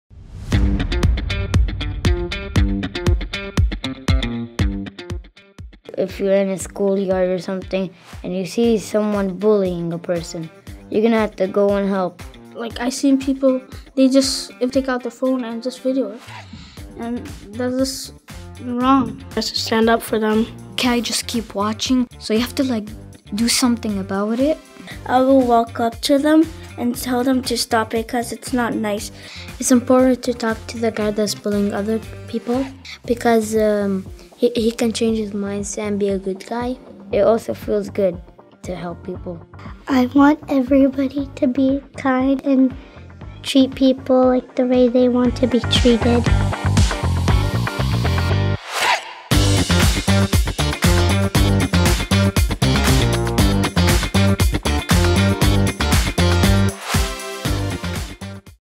Stand Up Against Bullying
Kids tell us how to stand up to a bully.